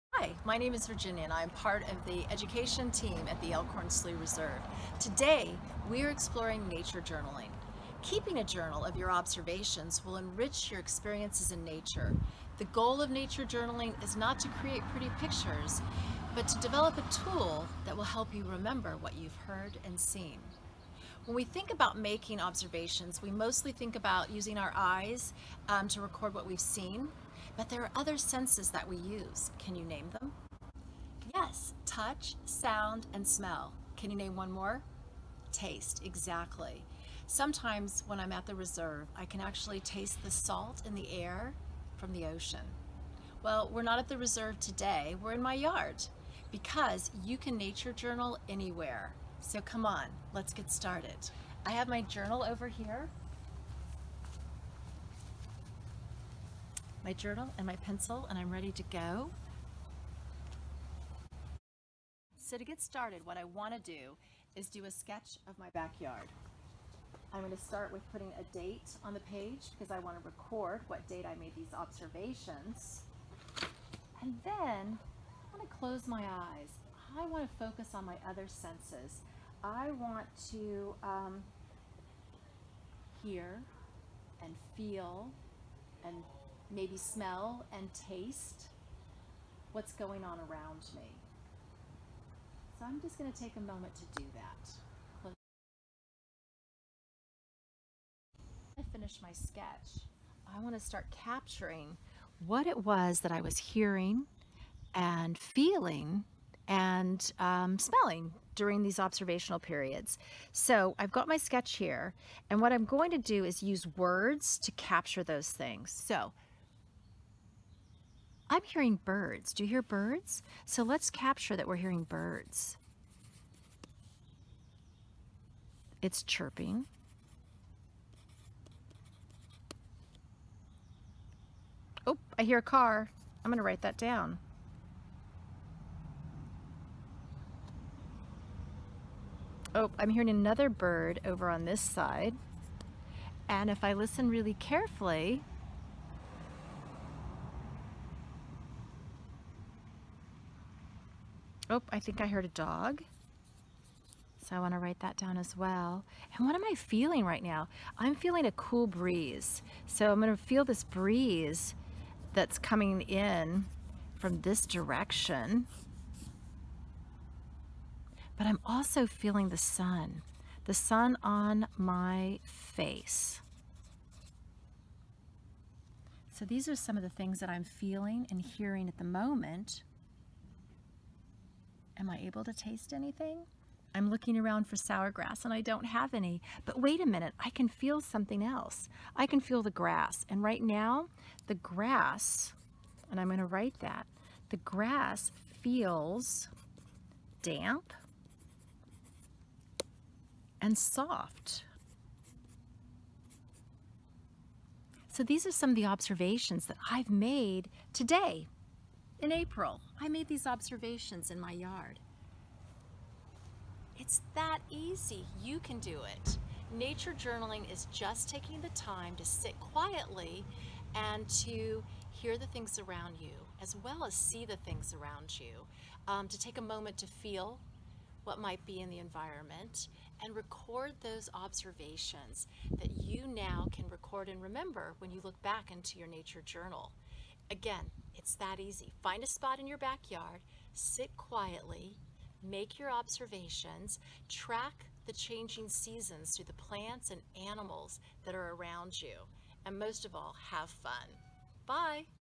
Journal in Place: Using Our Senses
In the third installment of "Journal in Place," a nature journaling project from the Elkhorn Slough Reserve, the Reserve's Education staff will help you use your senses and create a "sensory map" of your backyard!